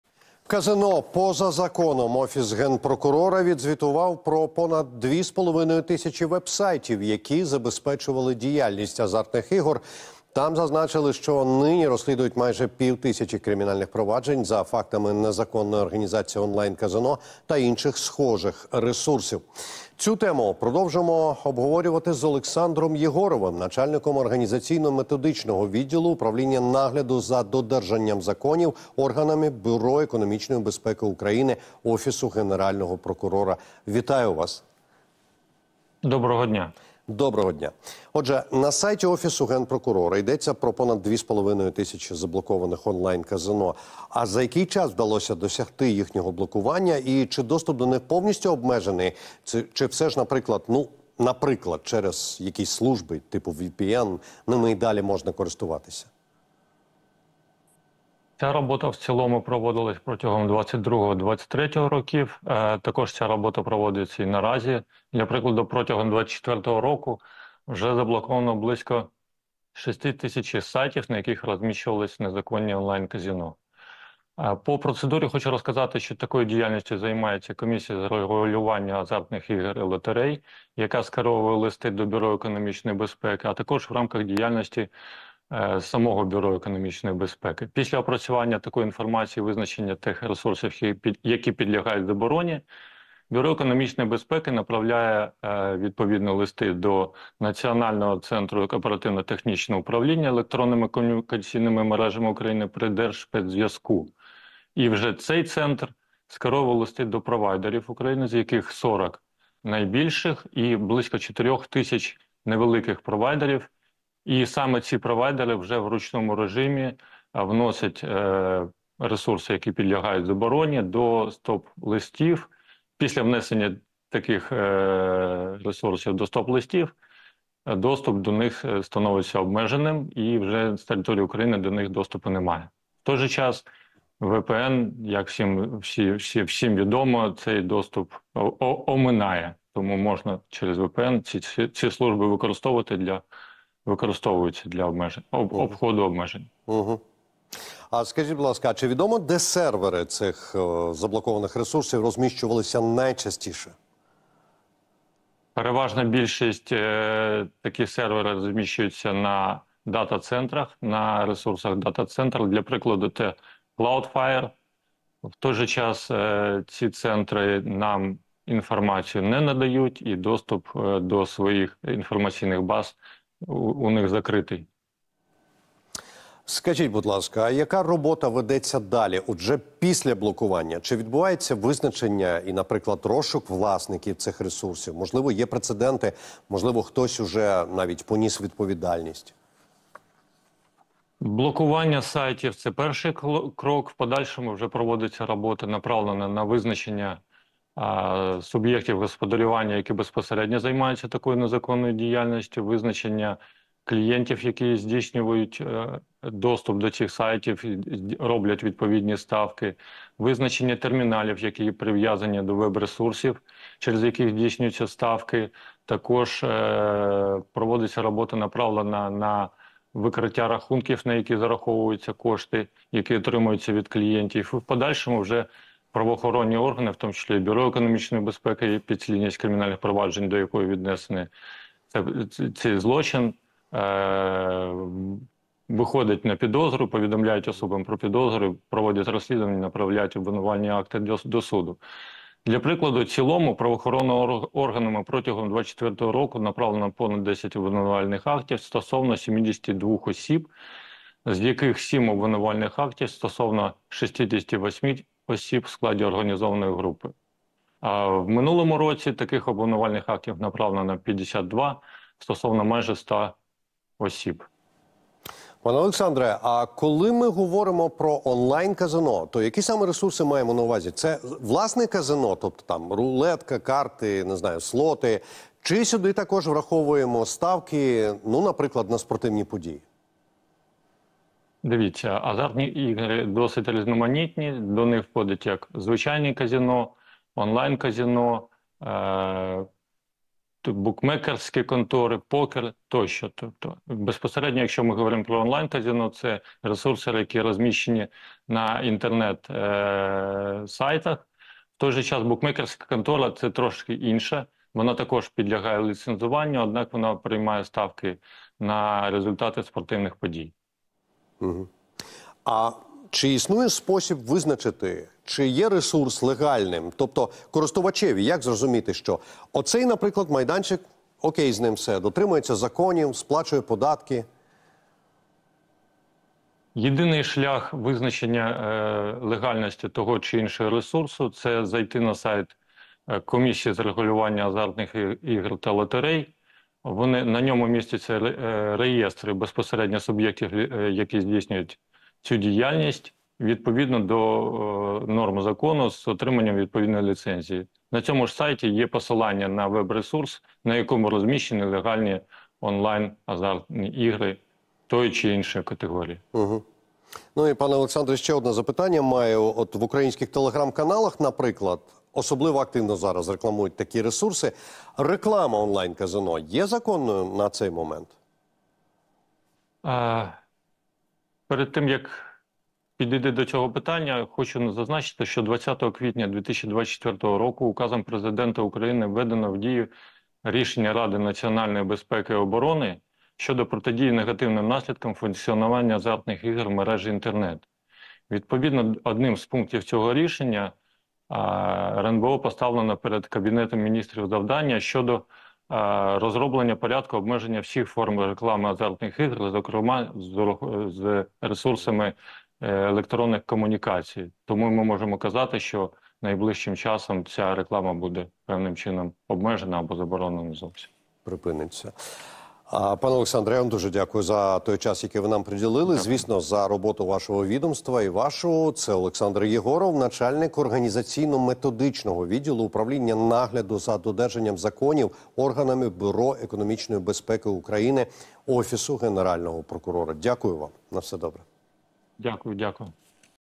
🚫ЗАБОРОНА ОНЛАЙН-КАЗИНО: в Офісі Генпрокурора розповіли процедуру розслідування нелегальних ресурсів
Протягом 2024 року в Україні заблоковано близько 6 тисяч сайтів онлайн-казино.
Про це у вівторок, 30 квітня, заявив Олександр Єгоров, начальник організаційно-методичного відділу управління нагляду за додержанням законів органами Бюро економічної безпеки України Офісу Генерального прокурора в ефірі телемарафону.